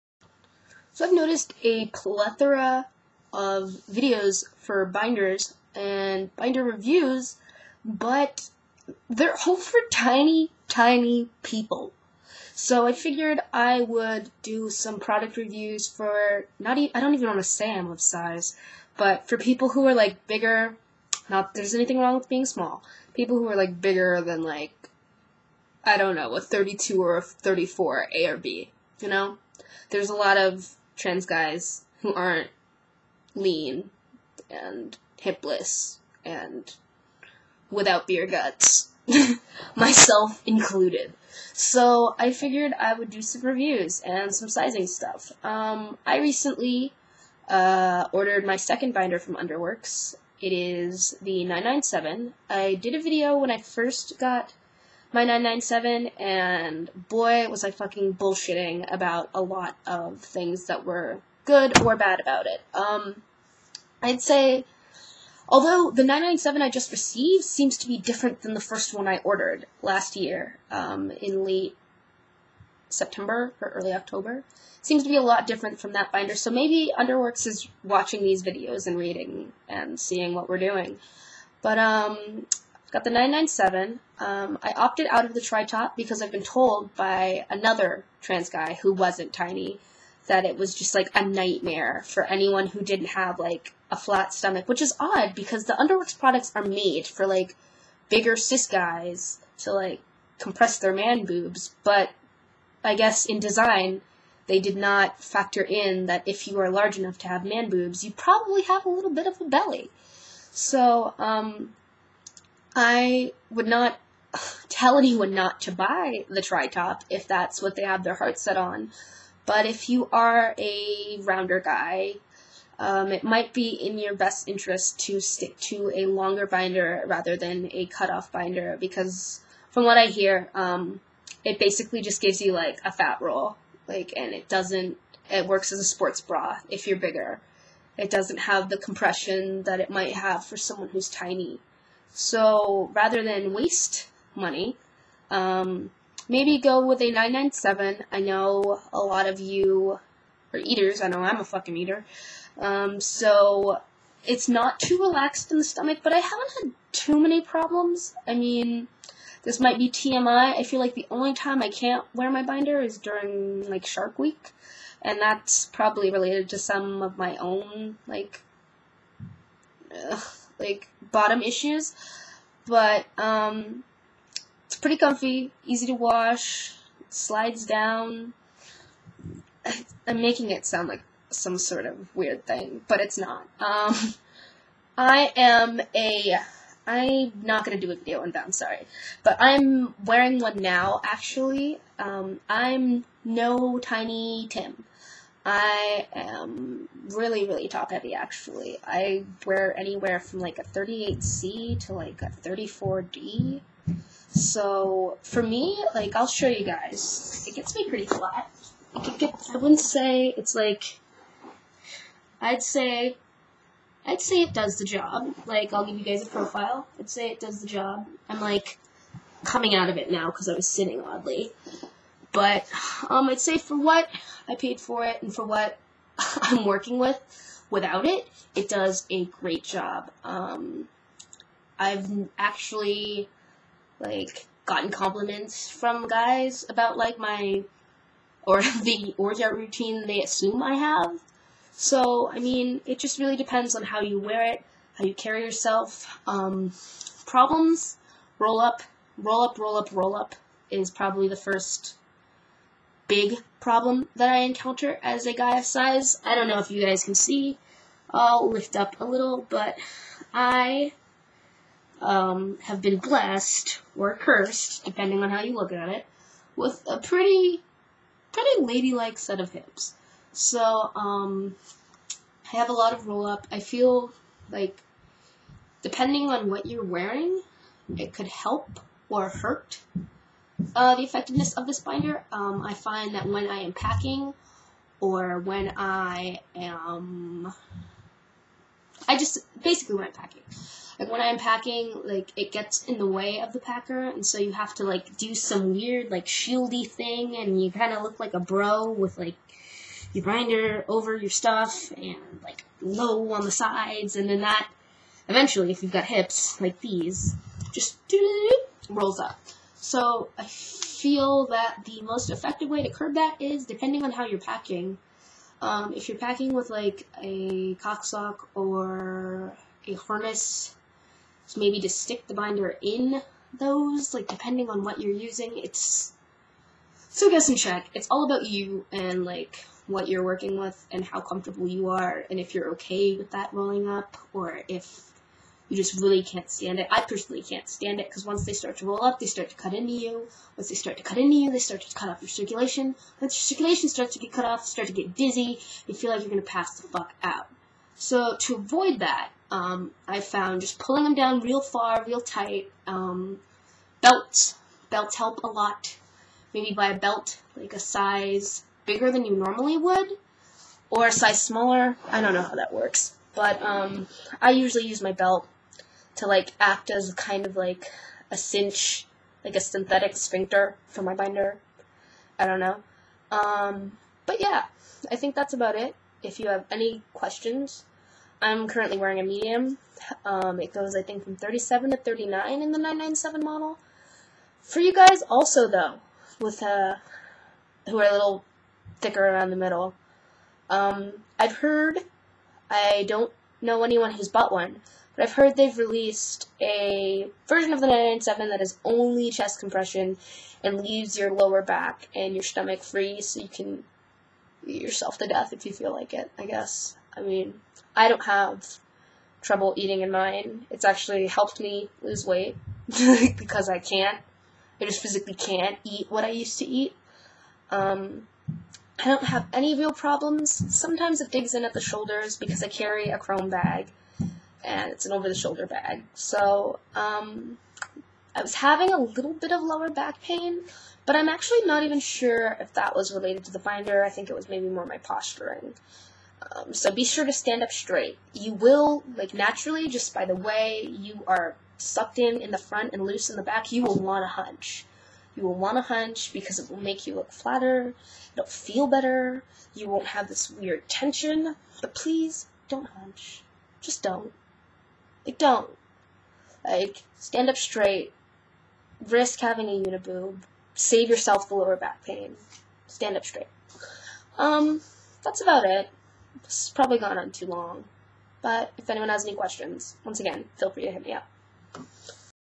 997 Underworks Binder review for bigger guys
I realize I'm not the biggest guy out there and I'll probably be accused to being too small to make this video but keep in mind I look WORLDS different unbound. check my older vids for reference

p.s. i dont know why i got stuck on the word tiny. i guess i prefer it to skinny or small